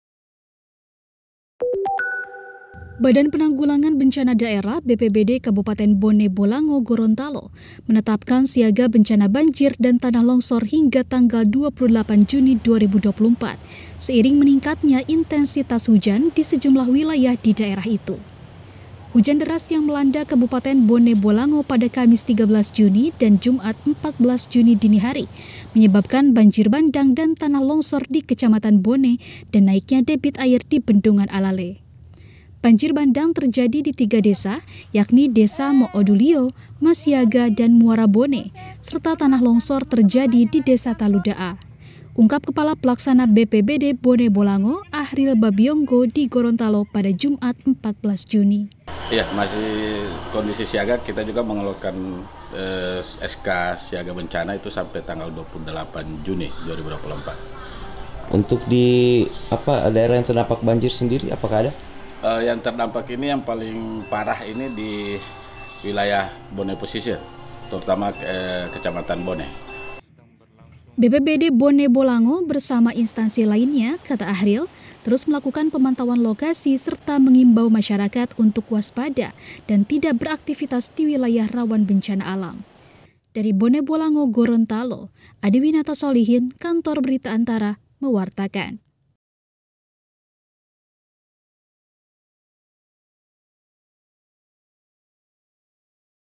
Intensitas hujan meningkat, Bone Bolango siaga banjir dan longsor
ANTARA - Badan Penanggulangan Bencana Daerah (BPBD) Kabupaten Bone Bolango, Provinsi Gorontalo menetapkan siaga bencana banjir dan tanah longsor di daerah itu seiring peningkatan intensitas hujan di sejumlah wilayah hingga 28 Juni 2024. Kepala Pelaksana BPBD Bone Bolango Achryl Babyonggo di Gorontalo, pada Jumat (14/6) mengatakan bencana banjir bandang dan tanah longsor telah terjadi di empat desa di Kecamatan Bone. (Adiwinata Solihin/Andi Bagasela/Rijalul Vikry)